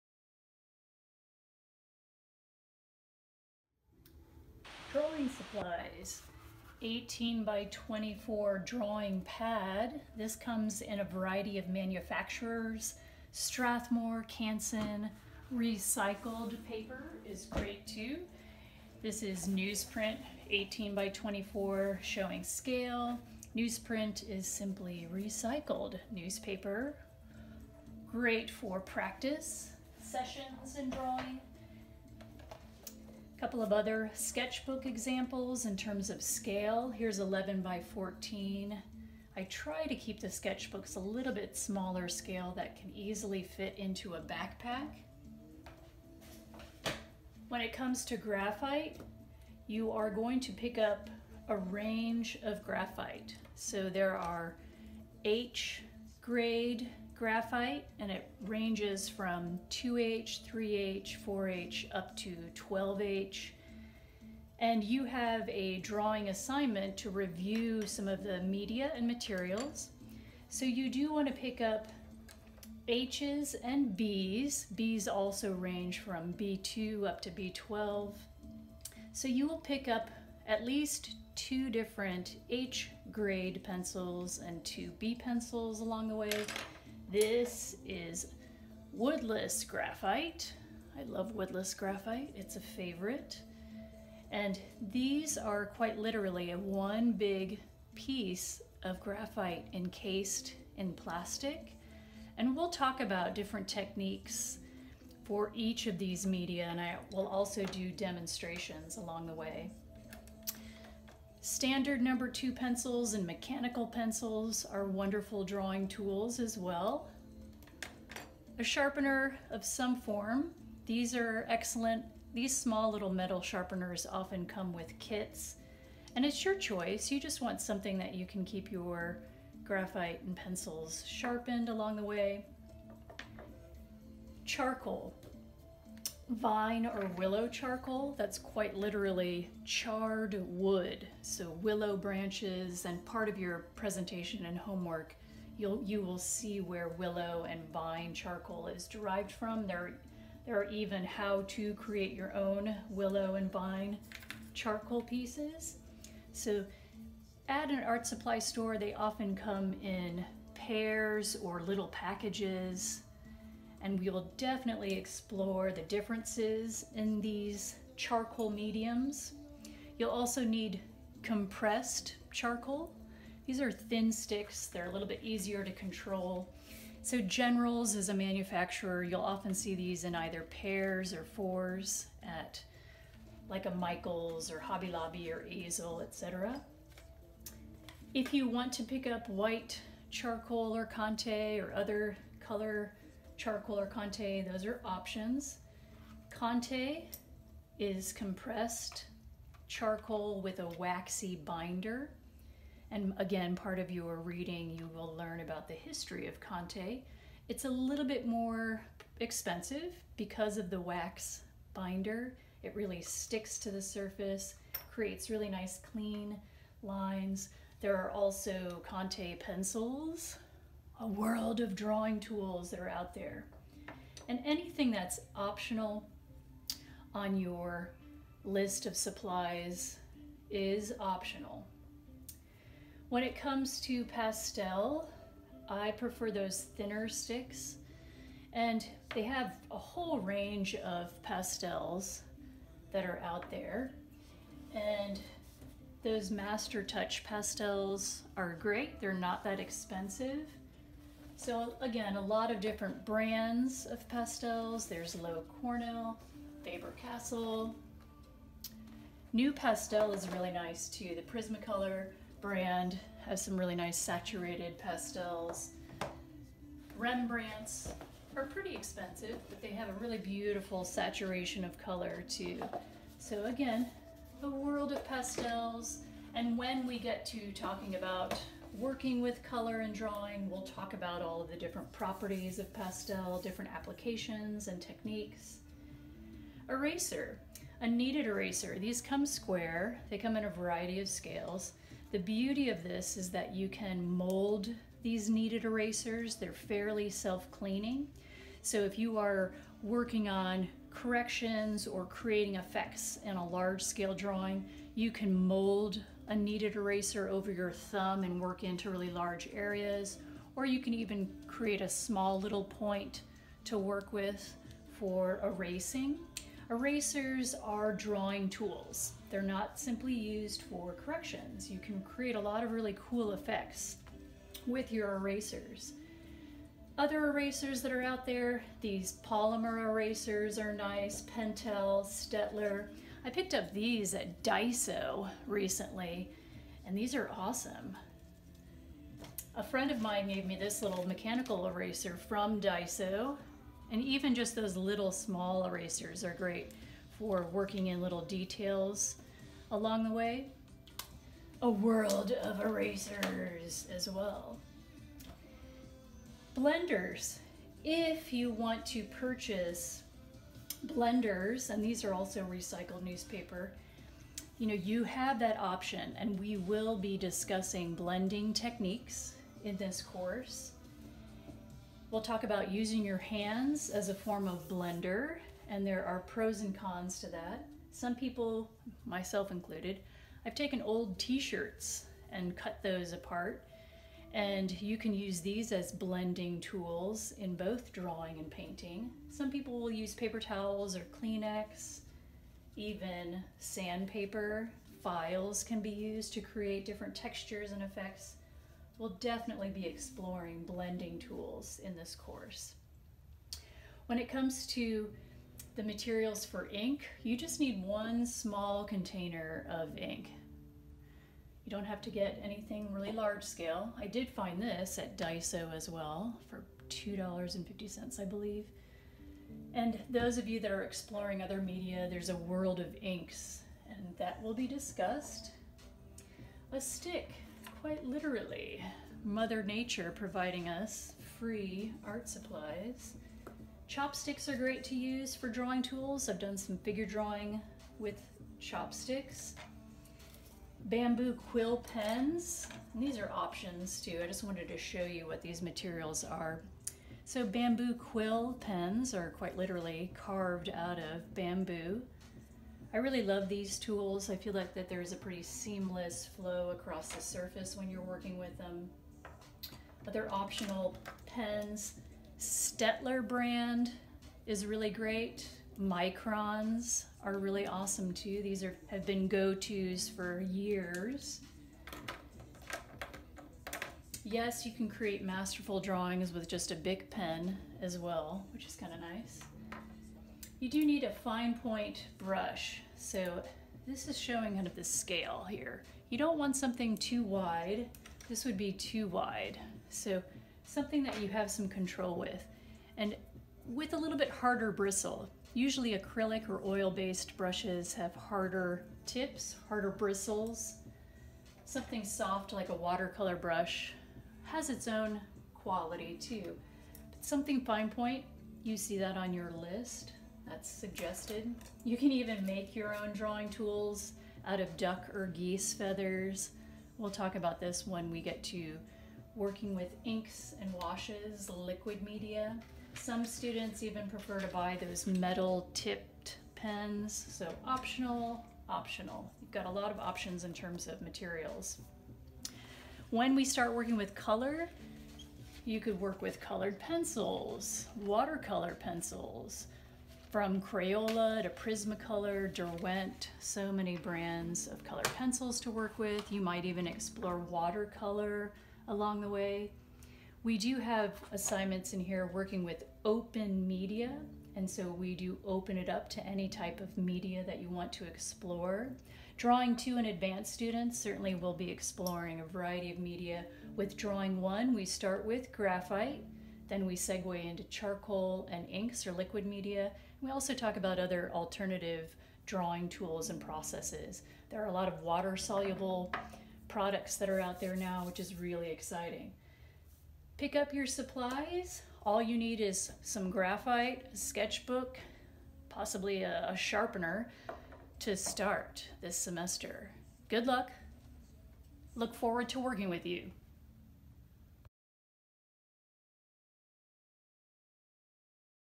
Intro to Drawing Supplies
Introduction to Drawing Supplies by professor Allison Gillies at North Lake College. Approximately 10 minutes sharing examples of Drawing media. ENJOY the Drawing Process!